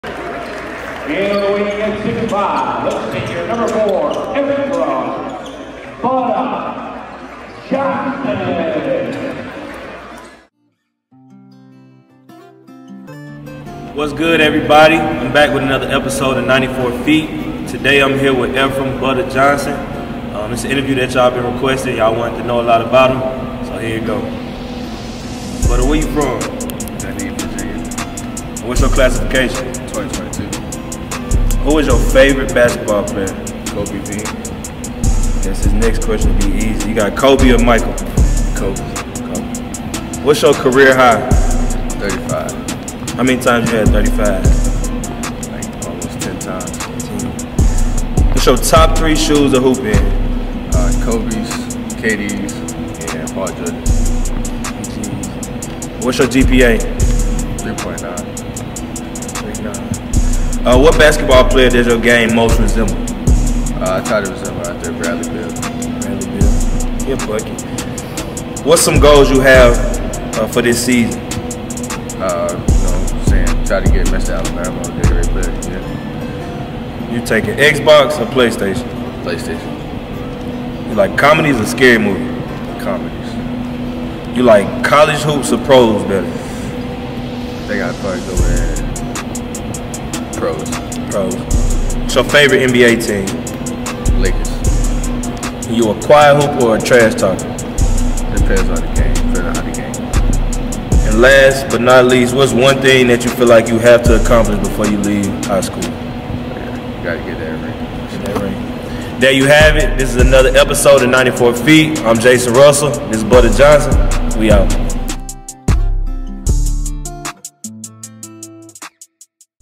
94 Feet- Episode 3: Efrem “Butta” Johnson
WE’RE BACK with Class of 2022 CG Butta Johnson as he goes 94 feet with @FF_FILMS to answer some questions about what he likes to do on and off the court. This is just Episode 3.